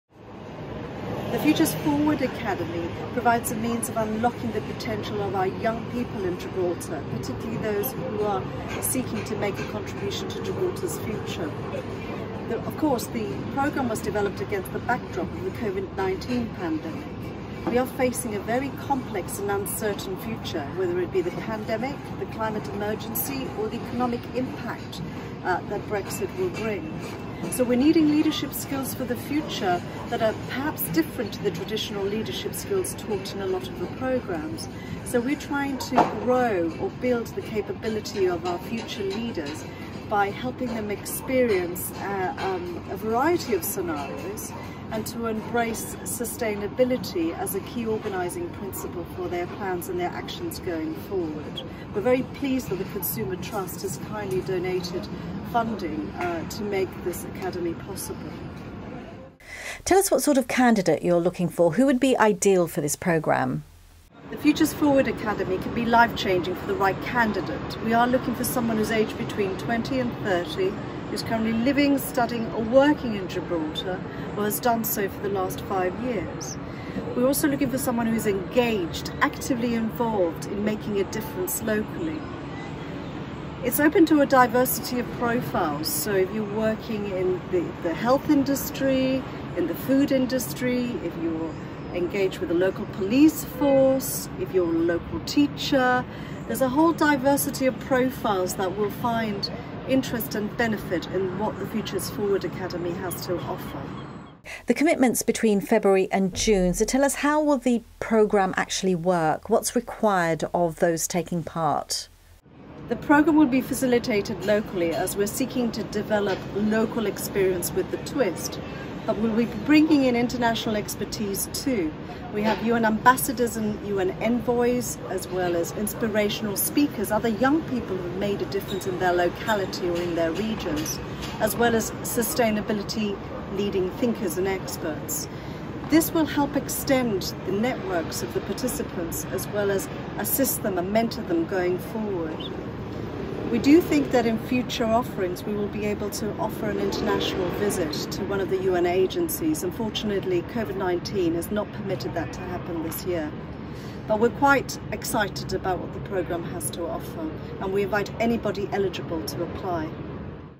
Initiative for young professionals to share & develop sustainable initiatives & ideas announced
An initiative for young professionals to share and develop sustainable initiatives and ideas has been announced by the Government. The Futures Forward Academy has been developed against a backdrop of the health crisis arising from the COVID-19 pandemic, economic challenges that arise from Brexit, as well as the climate emergency.

Those eligible for selection must be between 20 – 30 years old.  They will have to commit to being part of the programme from February until June.

Ros Astengo spoke to Professor Daniella Tilbury to find out more.